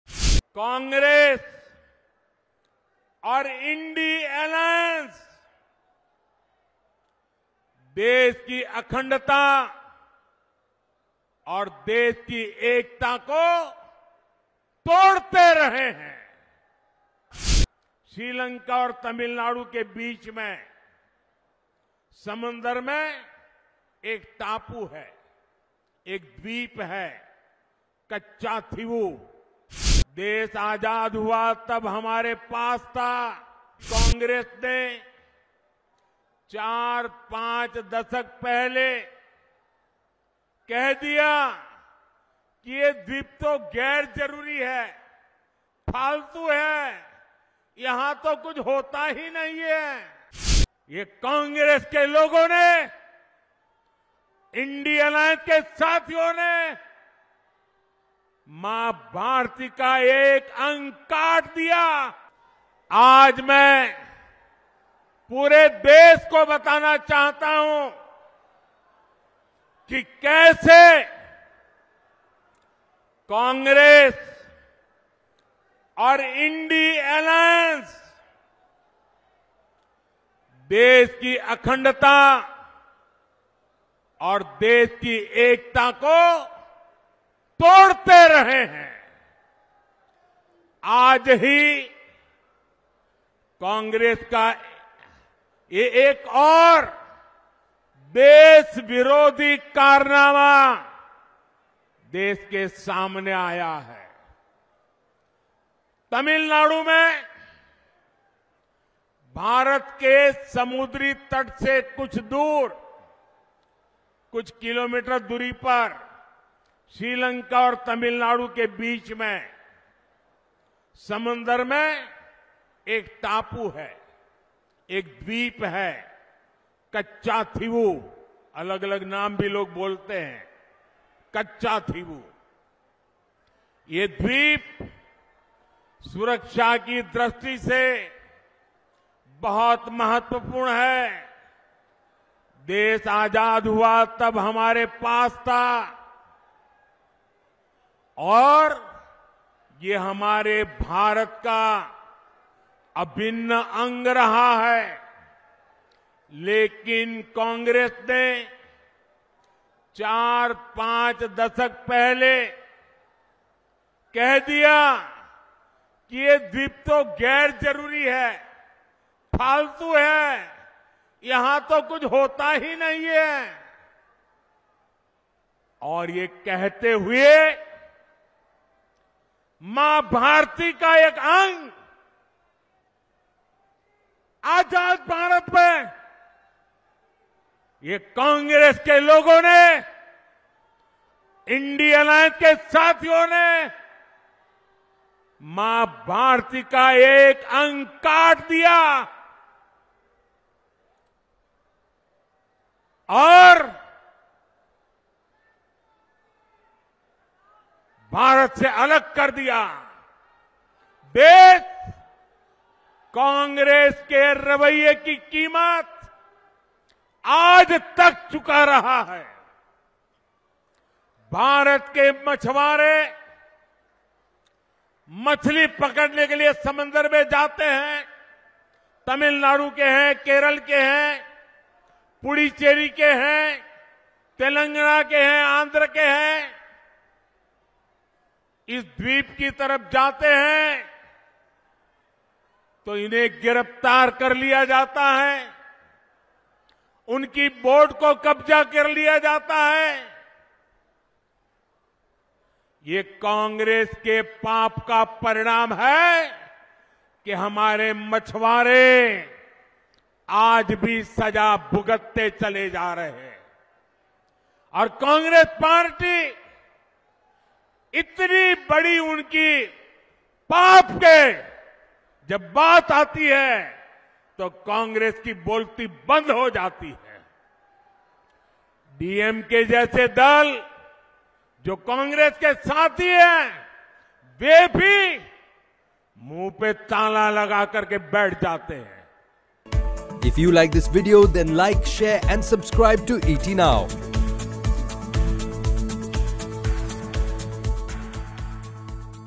“Maa Bharti Ka Ek Ang Kaat Diya…” PM Modi Blasts On Congress Over Katchatheevu Island | ET Now
Launching an attack on Congress over the Katchatheevu Island, PM Modi during a public meeting in Uttar Pradesh’s Meerut on March 31, PM Modi said that Congress and its INIDA bloc’s allies cut off a part of “Maa Bharti” and separated it from India.